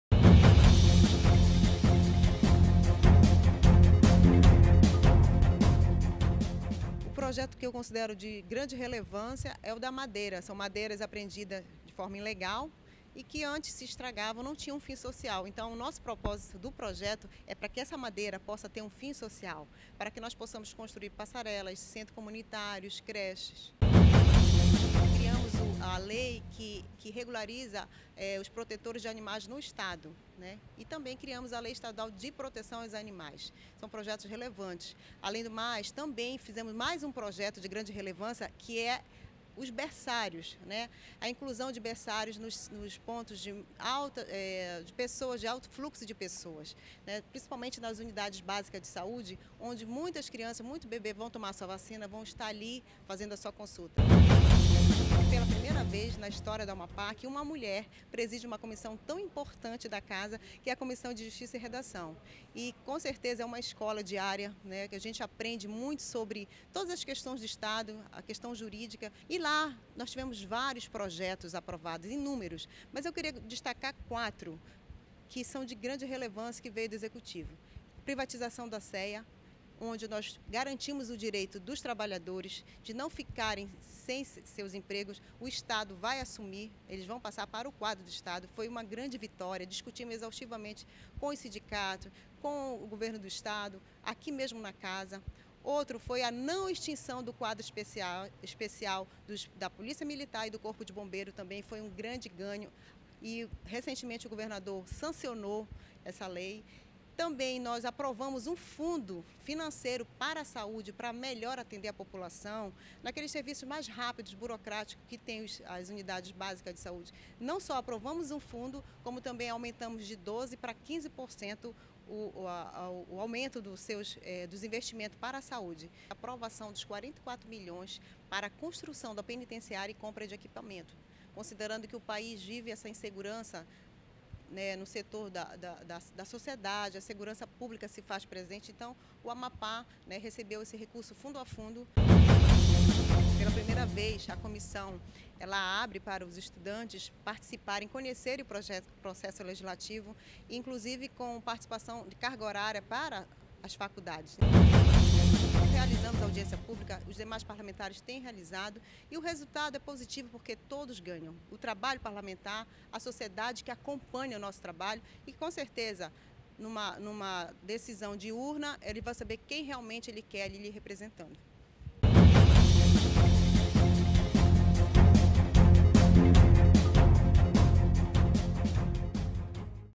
ATIVIDADE LEGISLATIVA
Deputada estadual Edna Auzier (PSD) faz um balanço das atividades, no primeiro semestre.